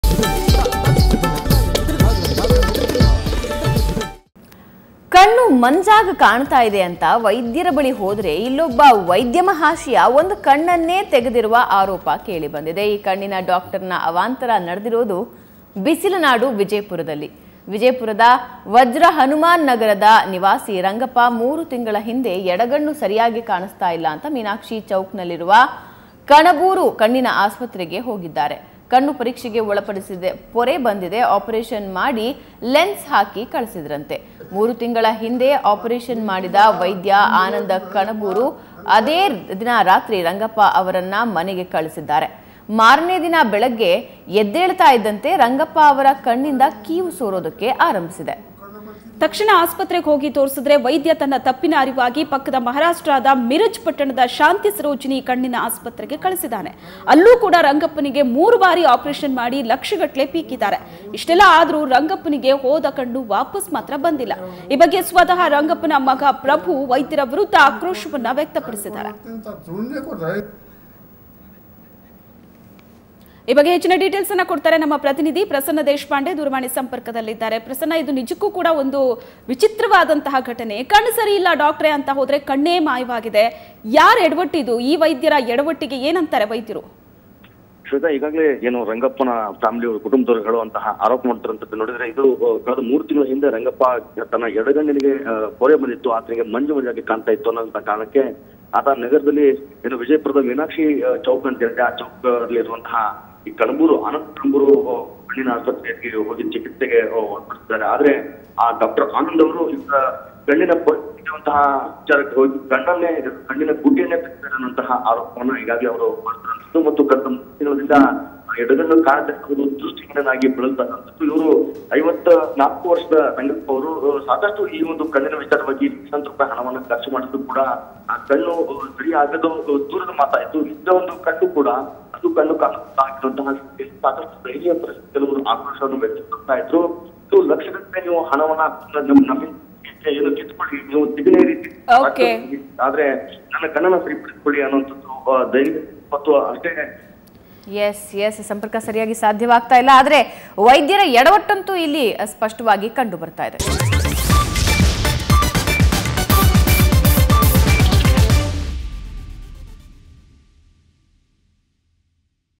Vijayapura : Man Lost Eye Due To Medical Negligence | ಎಡವಟ್ಟು ವೈದ್ಯನ ವಿರುದ್ಧ ಆಕ್ರೋಶ..!
Man Lost Eye Due To Medical Negligence In Vijayapura.

 ಎಡವಟ್ಟು ವೈದ್ಯನ ವಿರುದ್ಧ ಆಕ್ರೋಶ.